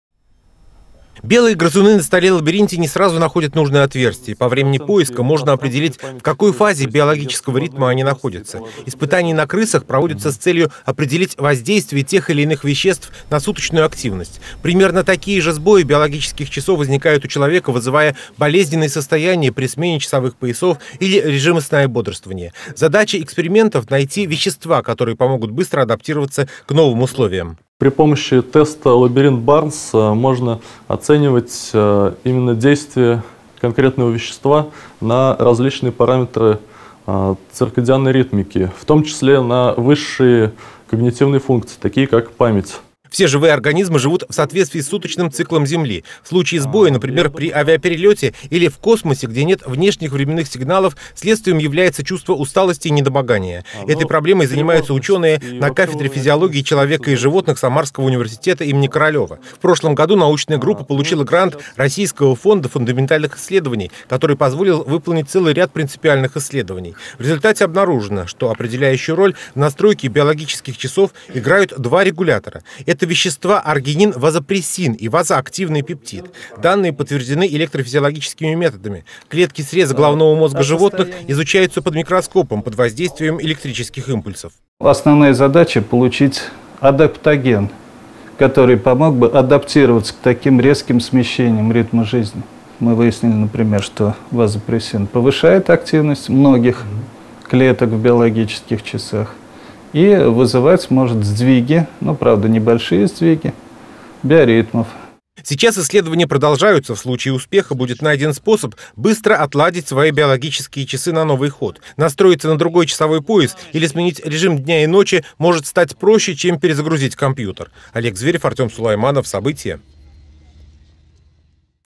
Перезагрузка на грызунах. В Самарском университете разрабатывают новые способы коррекции биоритмов
Настроить организм на точное время. Ученые нашли способ безболезненной настройки биоритмов. В век технического прогресса такая необходимость возникает при частых перелетах, работе по сменам и стрессах.  Научная группа Самарского университета проводит исследования, как быстро перенастроить биологические часы человека на те условия, в которых он оказался. Эксперименты учёные проводят  на подопытных животных, но уверены, что их открытия помогут людям быстро адаптироваться, а значит и сохранить здоровье.